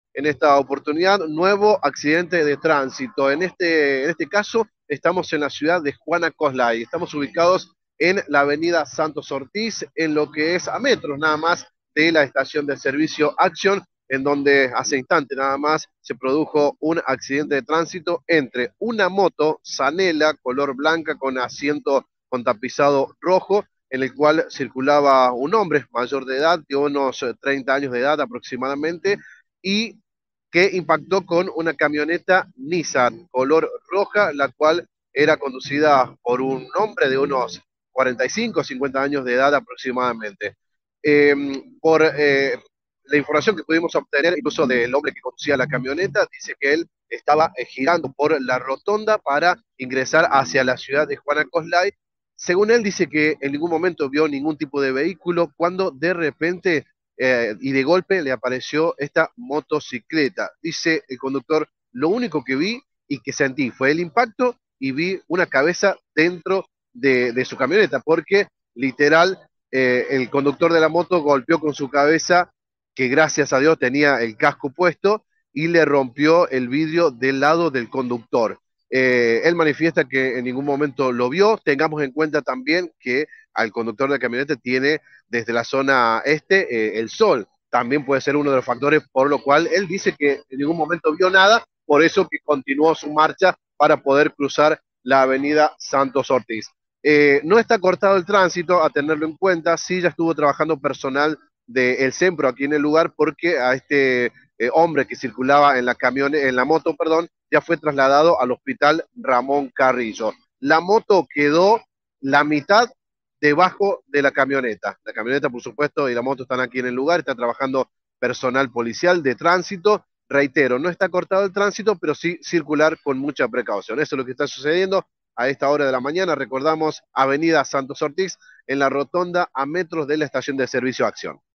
Accidente de tránsito en la Ciudad de Juana Koslay, más precisamente, en Avenida José Santos Ortiz
El siniestro ocurrió a metros de la estación de servicio Axion Energy, el accidente fue entre una moto Zanella blanca y una camioneta Nissan roja. La camioneta era conducida por un hombre de uno 60 años de edad aproximadamente, el cual era el único ocupante del vehículo.
En la moto iba un hombre de unos 35 años de edad.